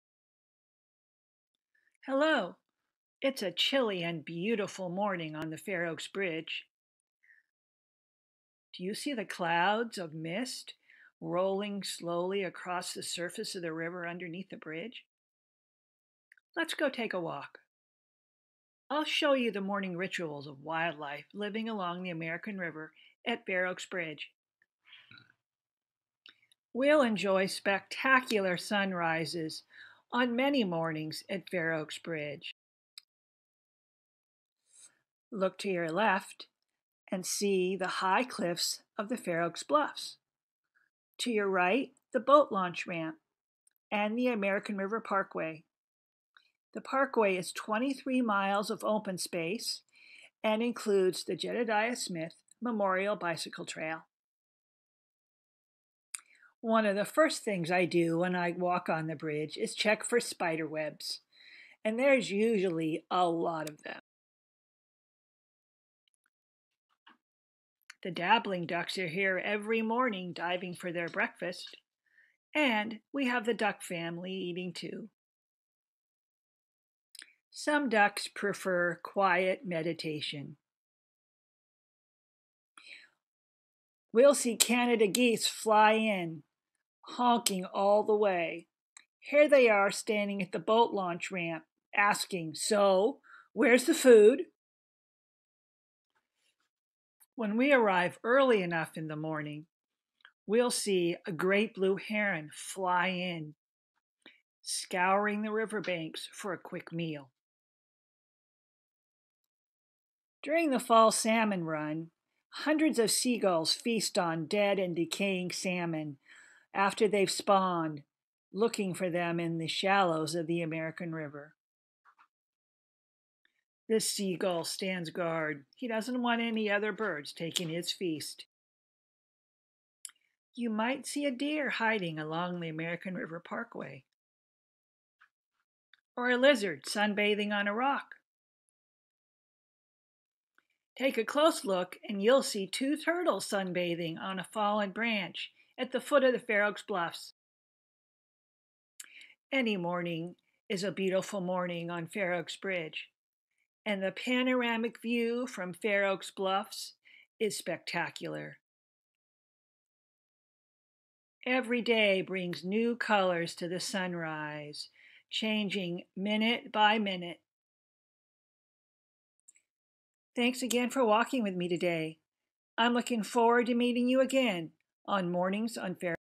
Fair Oaks Bridge and the American River Parkway
Walk with Janice as she describes early morning experiences on the American River at the Fair Oaks Bridge crossing. See the wakeup rituals of ducks and Canada Geese, scenic views and stunning sunrises.
Read blogs of first person observations and view photos of mornings at and nearby Fair Oaks Bridge on the American River